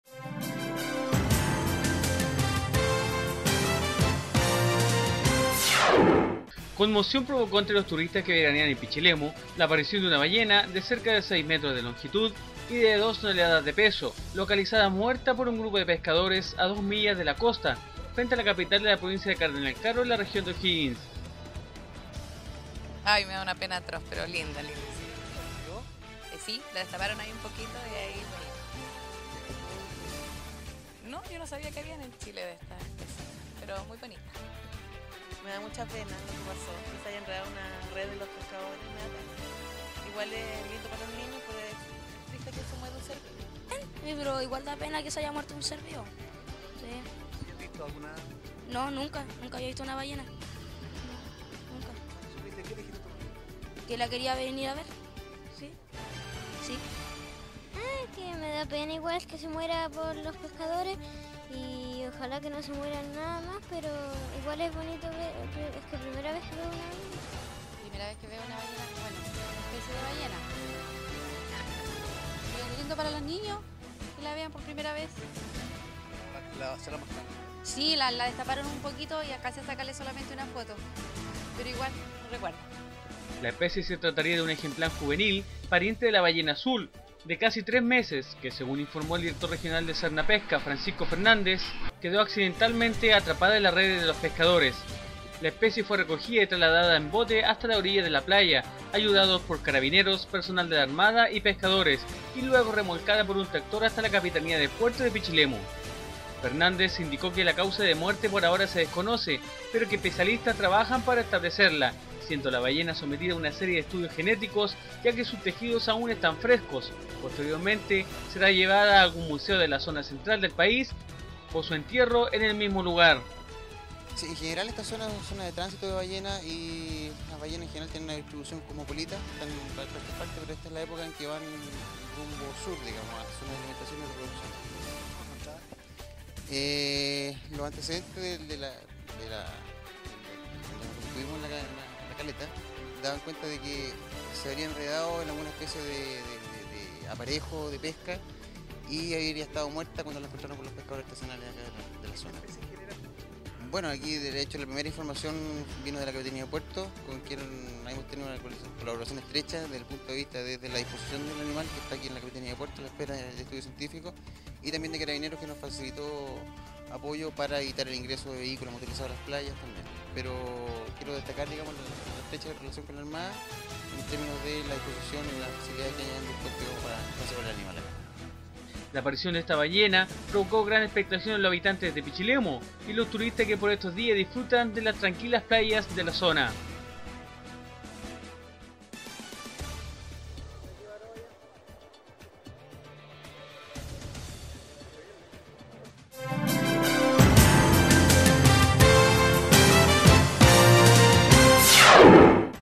Ballena aparece en Pichilemu
Encontrada muerta por unos pescadores de la zona, enredada en unos aparejos de la playa